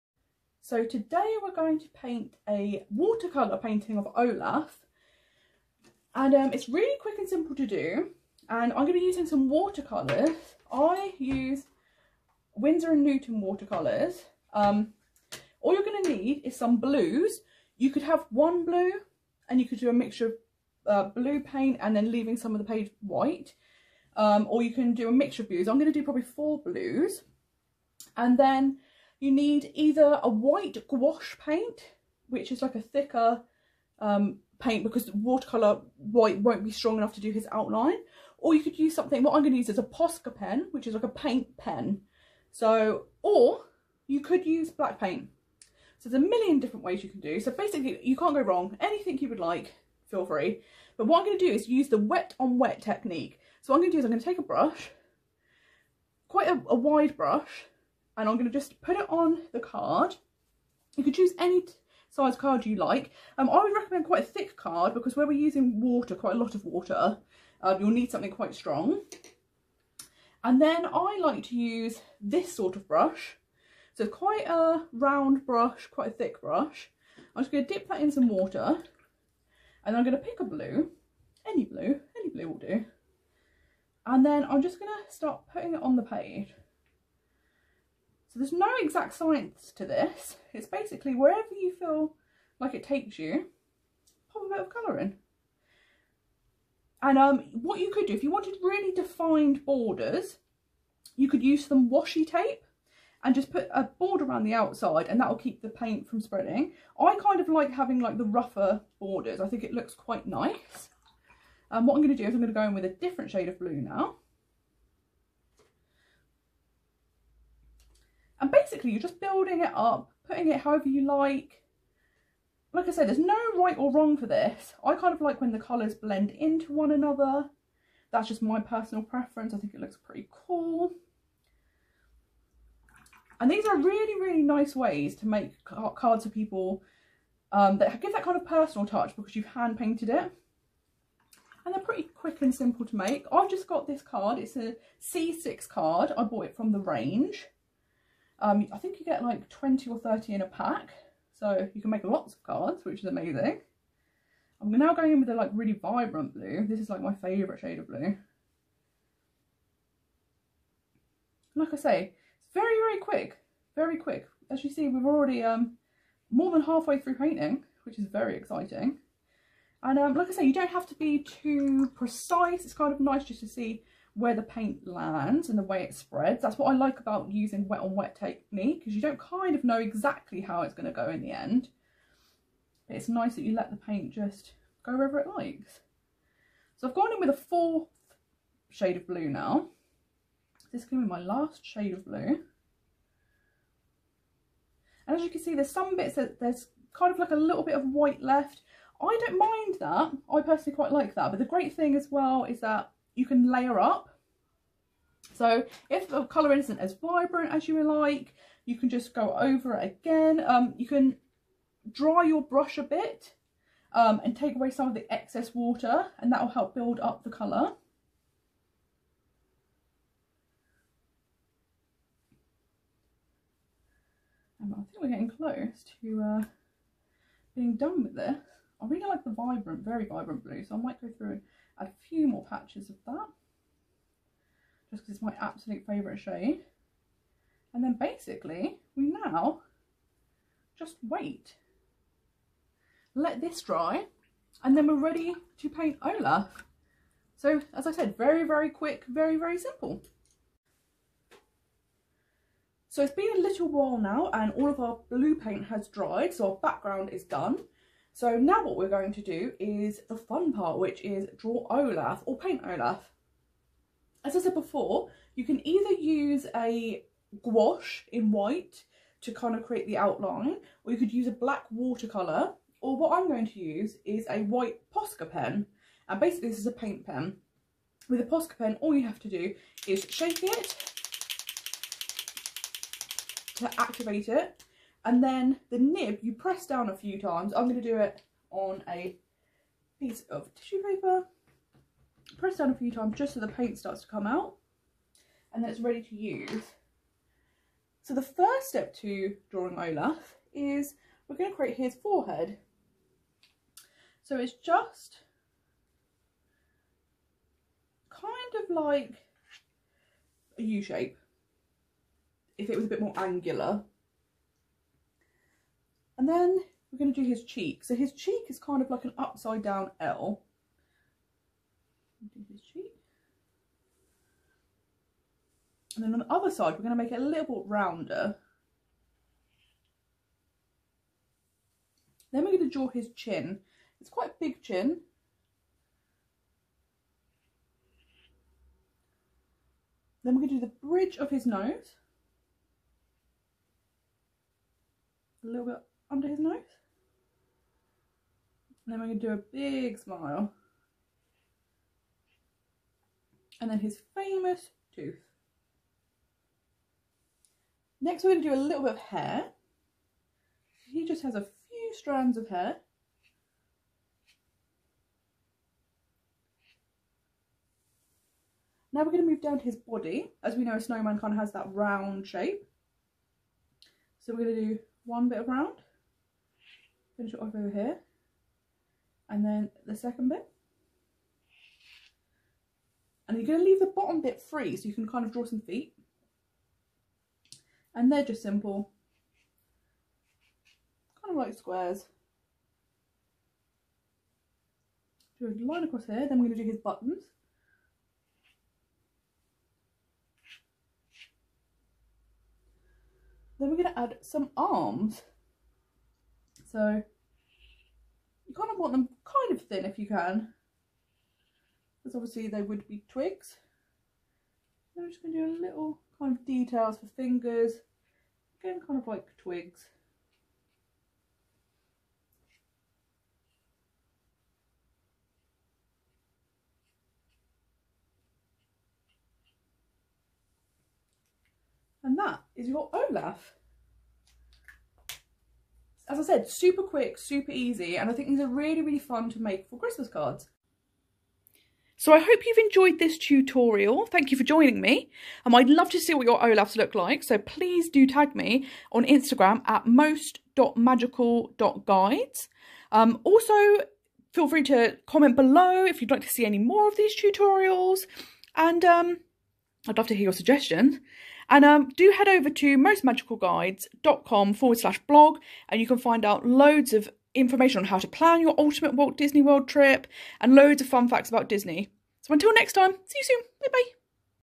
Super Easy Olaf from Frozen Christmas Card Tutorial!
For all you Frozen fans, or anyone wanting to make a Christmas card with a personal touch, here is a super easy tutorial on how to paint an Olaf Christmas card!

All you need is a blank card, some water colour paints, a paint brush, some water and a white paint pen!

In less than 10 minutes you will have painted your own festive card, perfect to send this year!

I'd love to see how your cards turned out!

If this has got you in the Disney spirit head to my website for lots of tips on how to plan your own most magical trip to Disney World!